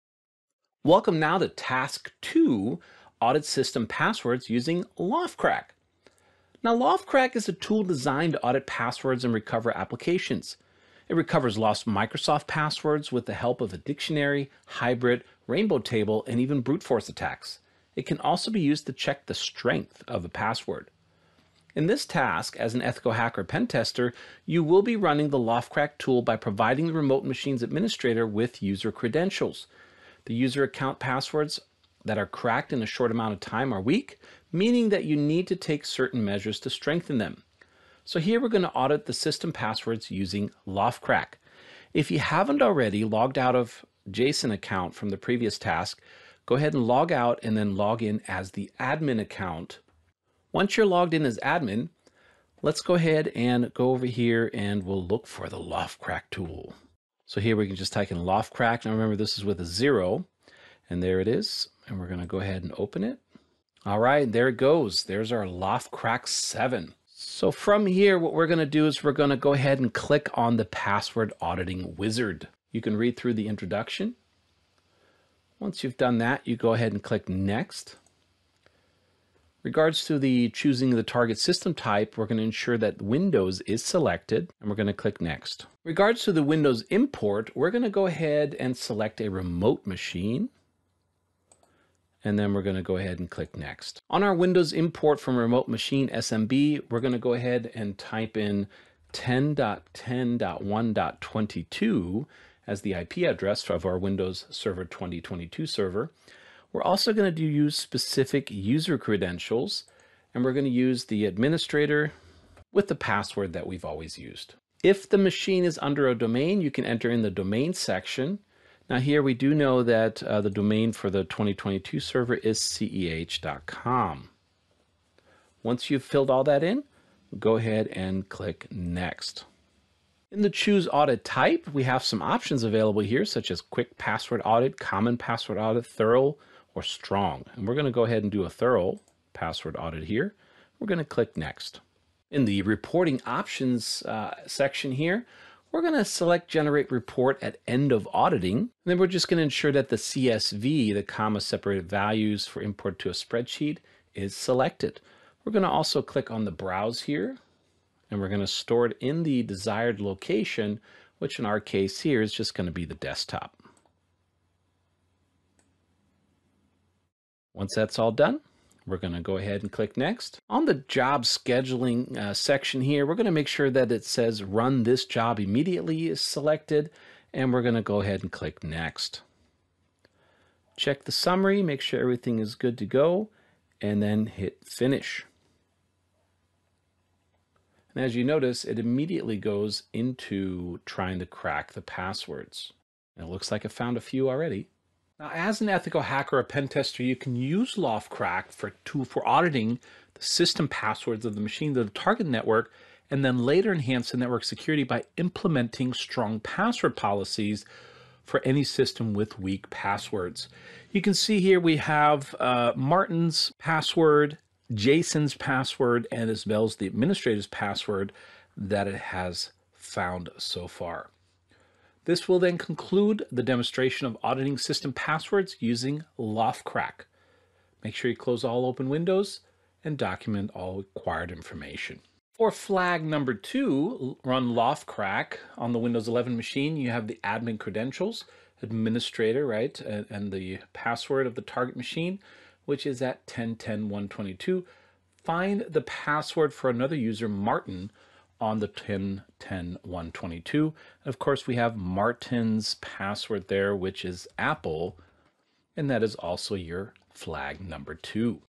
CyberQ CEHv12 Mod006 Lab01 Task 2 Audit System Passwords using L0phtCrack
L0phtCrack is a tool designed to audit passwords and recover applications. It recovers lost Microsoft Windows passwords with the help of a dictionary, hybrid, rainbow table, and brute-force attacks. It can also be used to check the strength of a password.
In this task, as an ethical hacker or penetration tester, you will be running the L0phtCrack tool by providing the remote machine’s administrator with user credentials. User account passwords that are cracked in a short amount of time are weak, meaning that you need to take certain measures to strengthen them.
Here, we will audit system passwords using L0phtCrack.